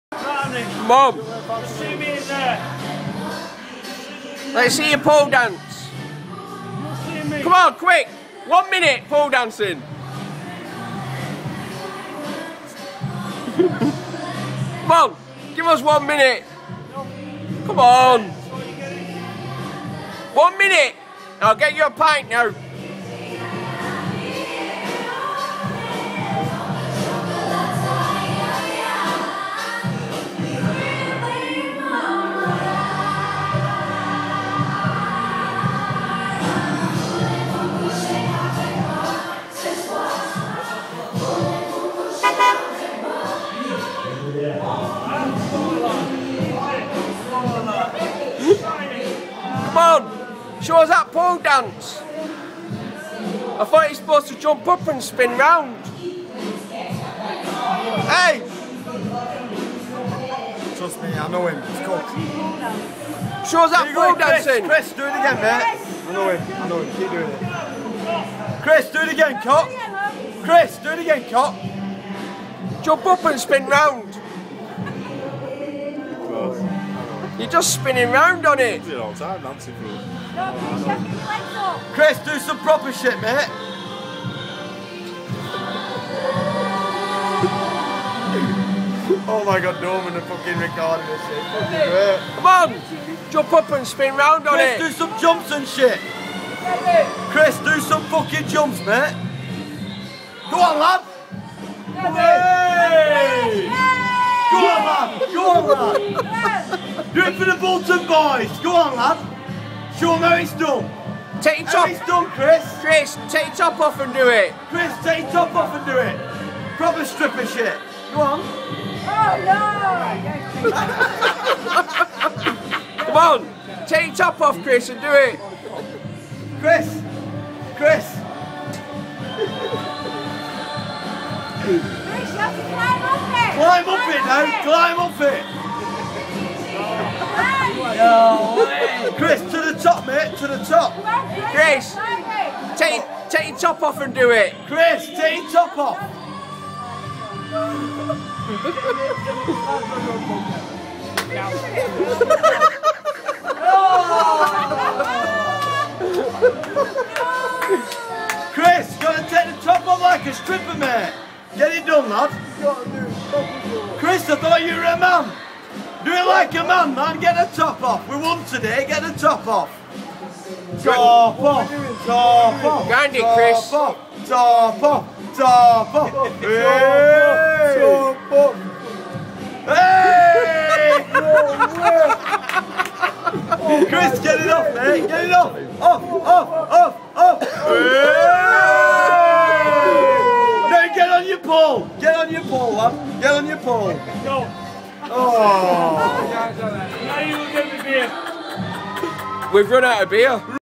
Bolton man who does pole dancin in the street
Drunken man pole dancing in the street